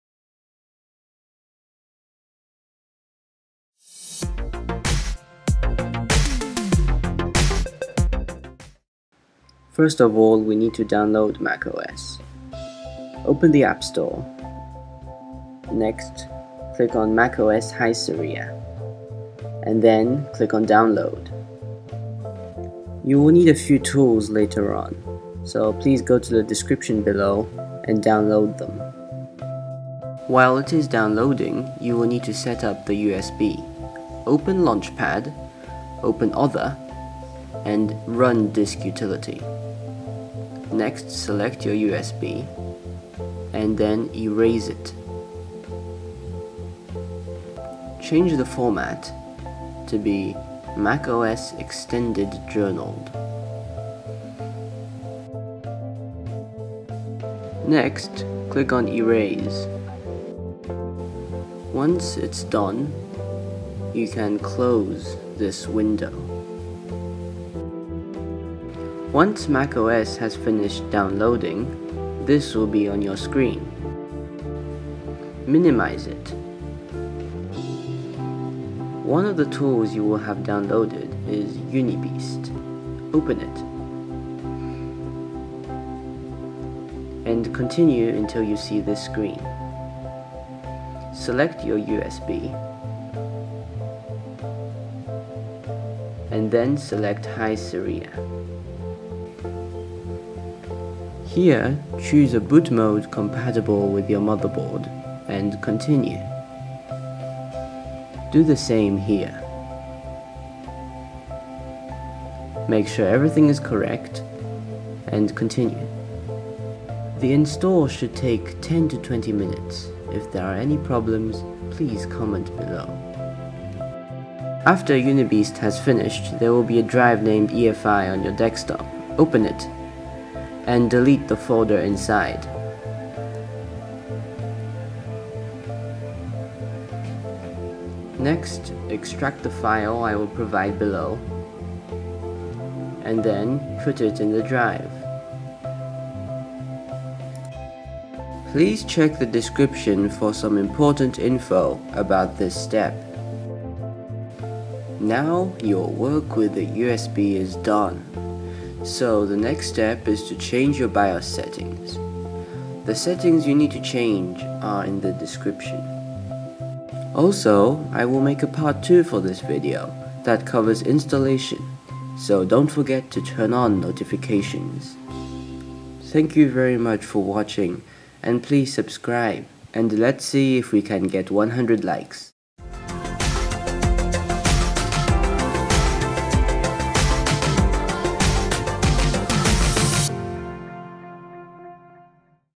How to make a Hackintosh on a COFFEE LAKE CPU || High Sierra || PART 1
OPEN ME

Tools you need:
 A 16GB or more USB
 A Mac
 Patience

Bios Settings:
 1. To access BIOS/UEFI Setup, press and hold Delete on a USB
 Keyboard while the system is booting up
 2. Load Optimized Defaults
 3. If your CPU supports VT-d, disable it
 4. If your system has CFG-Lock, disable it
 5. If your system has Secure Boot Mode, disable it
 6. Set OS Type to Other OS
 7. If your system has IO Serial Port, disable it
 8. Set XHCI Handoff to Enabled
 9. If you have a 6 series or x58 system with AWARD BIOS, disable
 USB 3.0
 10. Save and exit.

IMPORTANT NOTE
 In the EFI folder I will provide there will be a sub folder named
 Clover, inside
 there are 2 .plist files, rename one of the files (choose the one
 matching your CPU eg. If you have an i7 CPU then rename "i7-
 config.plist" to "config.plist").